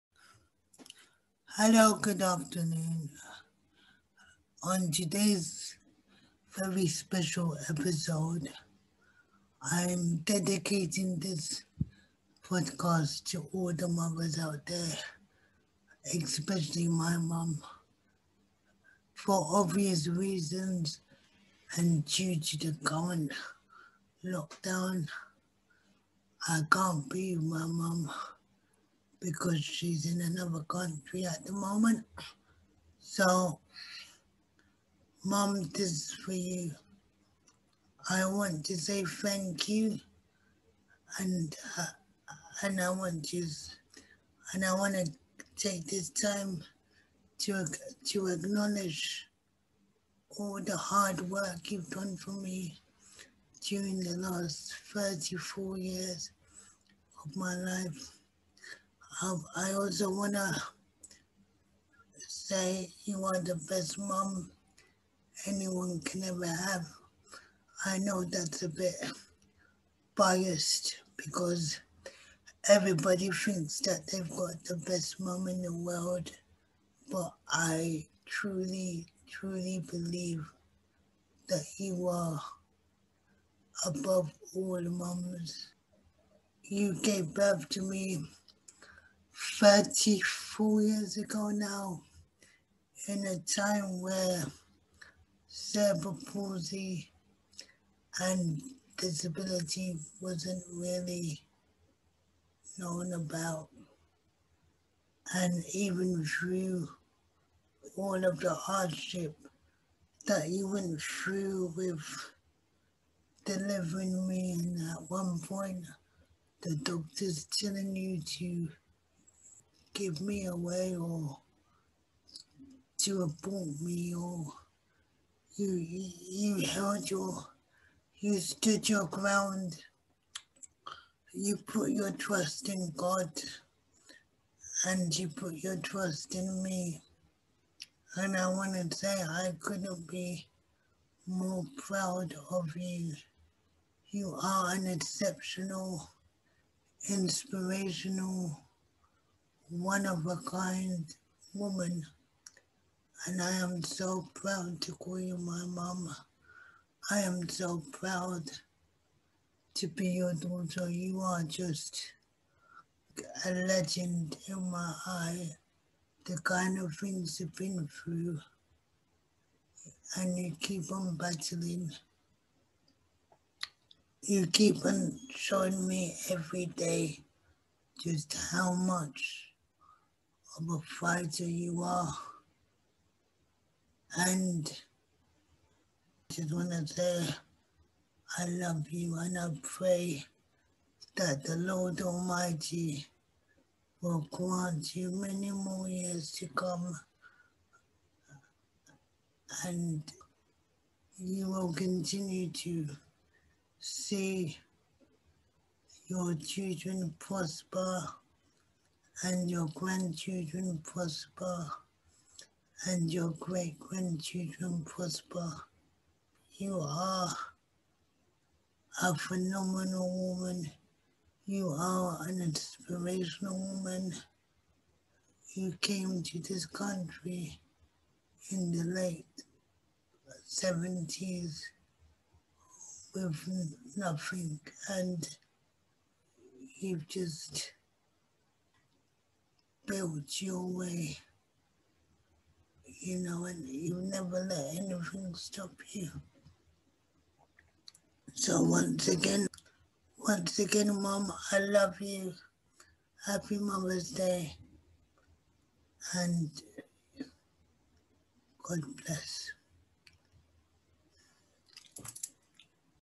To my phenomenal mum
on this very special Sunday in the UK as we're celebrating mothering Sunday and because of the current global situation I can't be with my mother.I wanted to share my love and gratitude to my mum for a phenomenal inspirational hard working God Fearing  Strong courageous African Mother